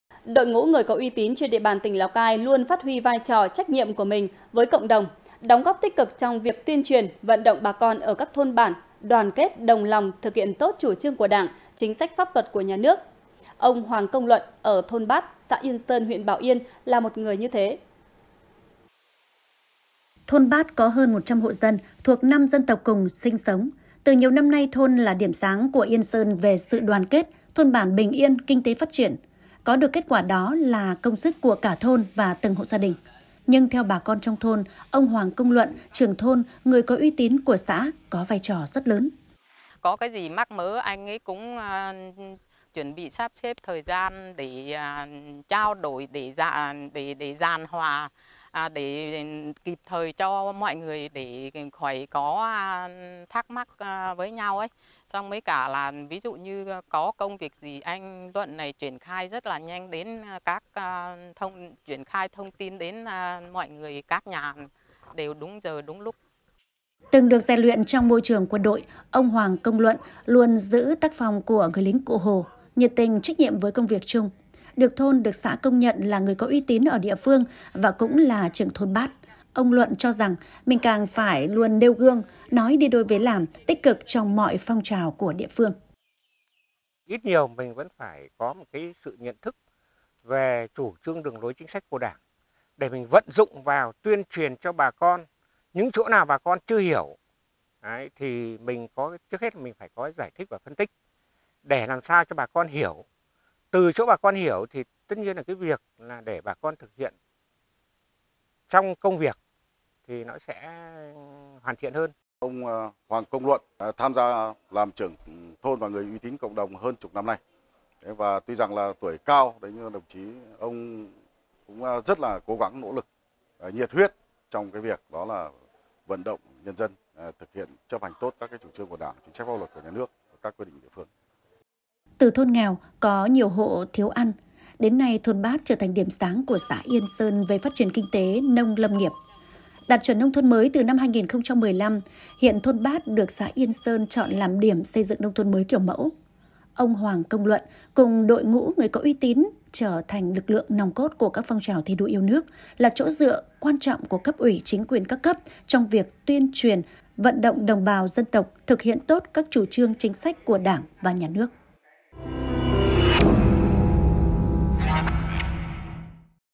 Người có uy tín - Cầu nối giữa ý Đảng - lòng dân | THLC
Laocaitv.vn - Đội ngũ người uy tín trên địa bàn tỉnh Lào Cai luôn phát huy vai trò, trách nhiệm của mình với cộng đồng, đóng góp tích cực trong việc tuyên truyền, vận động bà con ở các thôn bản đoàn kết, đồng lòng thực hiện tốt chủ trương của Đảng, chính sách pháp luật của Nhà nước. Ông Hoàng Công Luận ở thôn Bát, xã Yên Sơn, huyện Bảo Yên là một người như thế.

Đài Phát thanh - Truyền hình Lào Cai